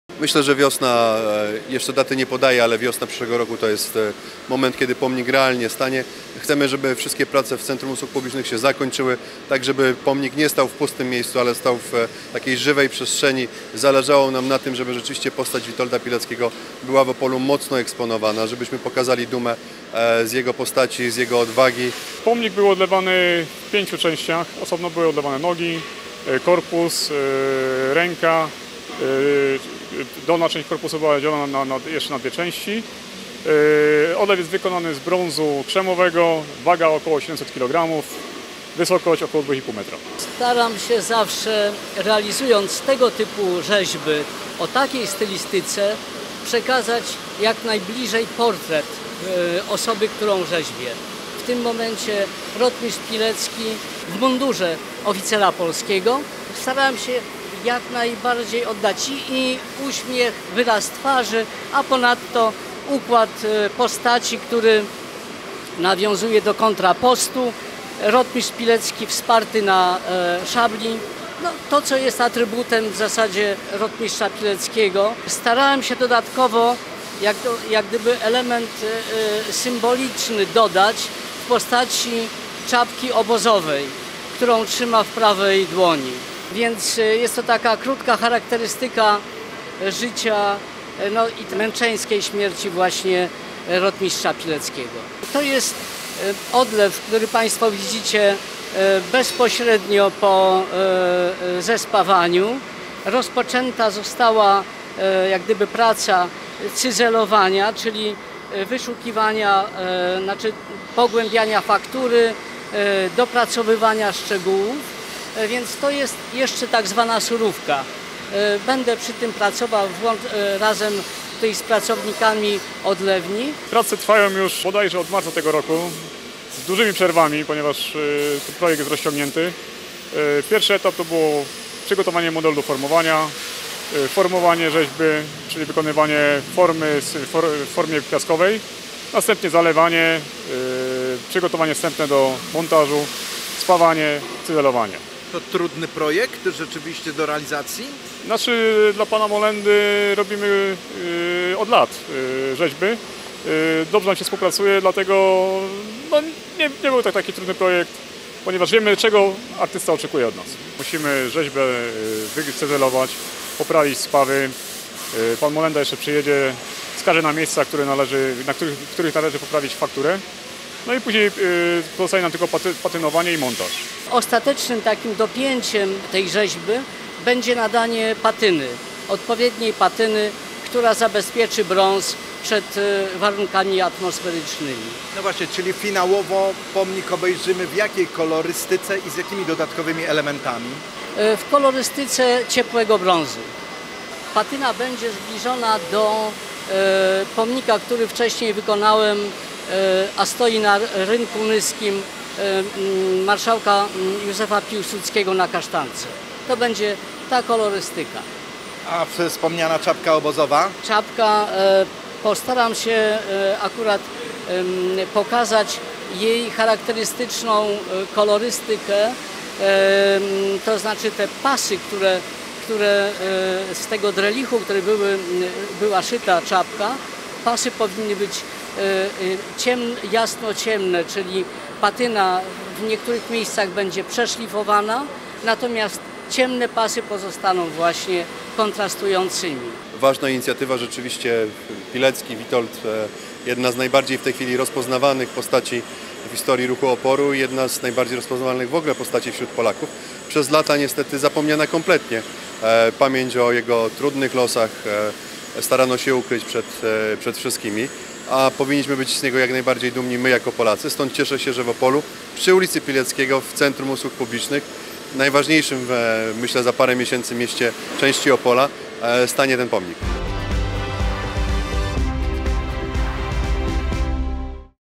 Pomnik rotmistrza Pileckiego aktualny stan prac
Późną jesienią powinny zakończyć się prace nad detalami a na wiosnę zostanie on postawiony przy ulicy jego imienia. Mowa o pomniku rotmistrza Witolda Pileckiego, który jest wykonywany w opolskiej pracowni Art Odlew. Za nami już główne prace przyznają zarówno autor projektu jak i wykonawcy.